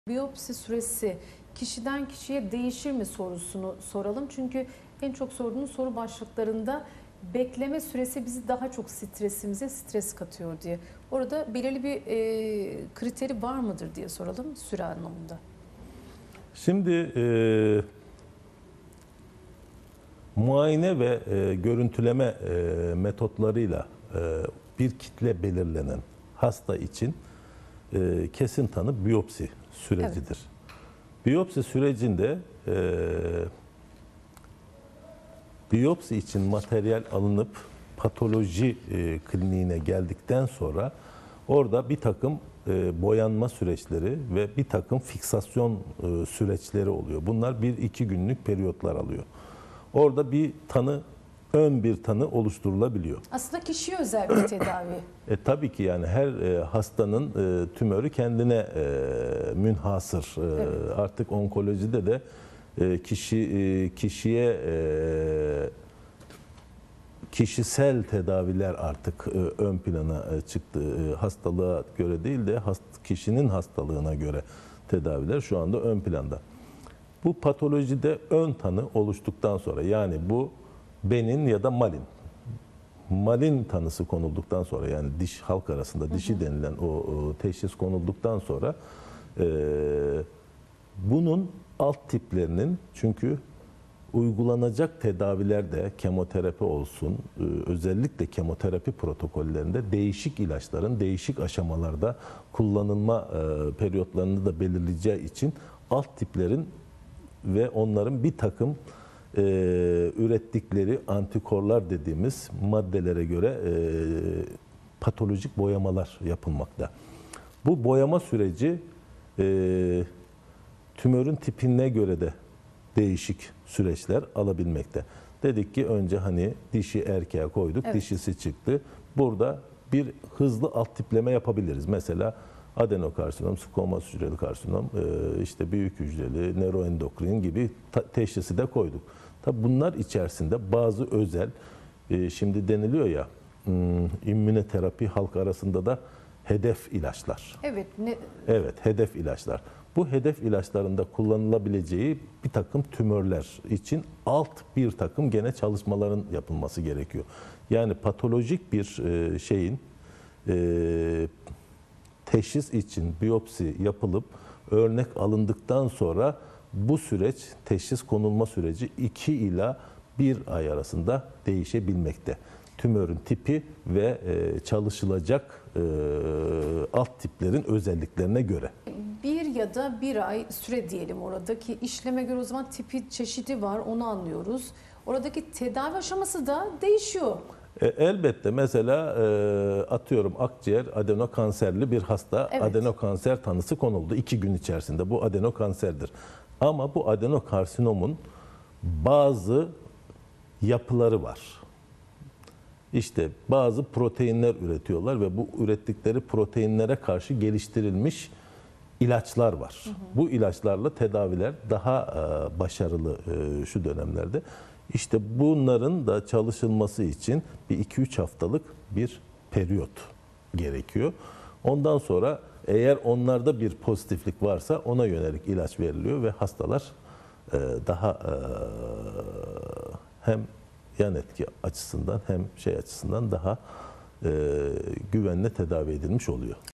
Radyasyon Onkoloji Uzmanı Prof. Dr. Vecdi Ertekin '' Kanser Tedavileri '' anlatıyor
Avrasya Hastanesi Radyasyon Onkoloji Uzmanı Prof. Dr. Vecdi Ertekin '' Kanser Tedavileri '' hakkında detayı bilgiler veriyor